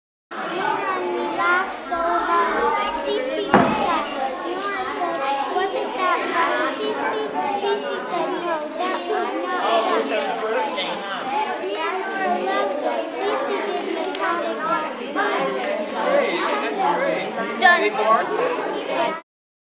Betty Snowden of the Georgetown Senior Center
Betty Snowden of the Georgetown Senior Center encourages a young girl in her reading. At St. John's Church in Georgetown.